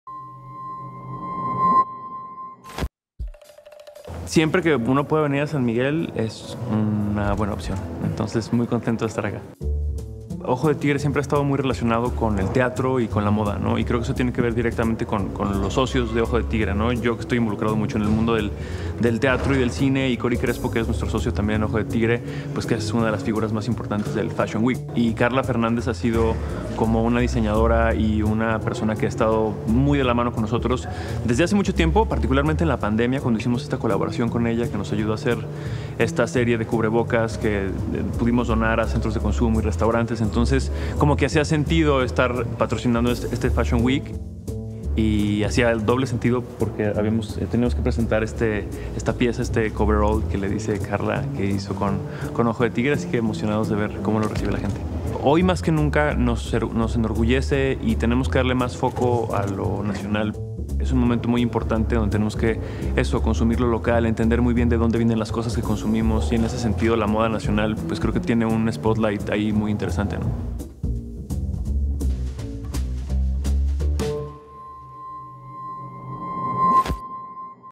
Luis Gerardo Méndez lleva el diseño que hará historia | EL MEZCAL DE LA MODA por Mezcal Ojo de Tigre
Luis Gerardo Méndez, actor mexicano y creador de Mezcal Ojo de Tigre, habla sobre una gran colaboración con Carla Fernández.